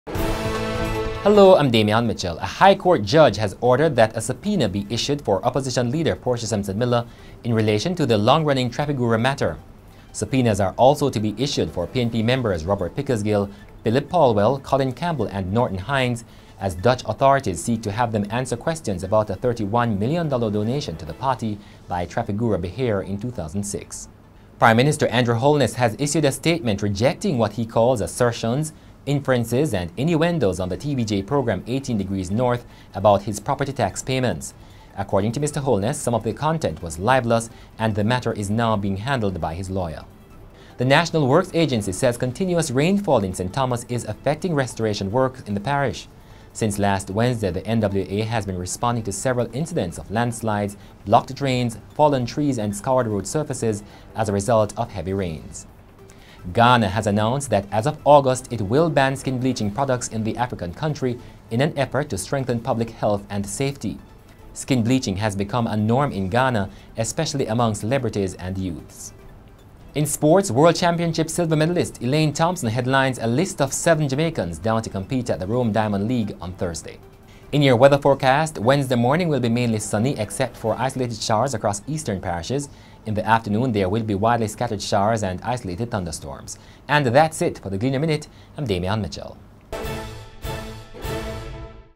THE GLEANER MINUTE: Portia subpoenaed ... Holness property tax ... Ghana to ban bleaching
MAY 31, 2016: Judge has orders subpoena be issued for Portia Simpson Miller in relation to the long-running Trafigura matter ... Andrew Holness has issued a statement rejecting what he calls assertions, inferences and innuendos on the TVJ programme, '18 Degrees North' ... NWA says continuous rainfall in St Thomas is affecting restoration work in the parish ... Ghana has announced that as of August, it will ban skin bleaching products ... IN SPORTS: Elaine Thompson headlines a list of seven Jamaicans down to compete at the Rome Diamond League on Thursday.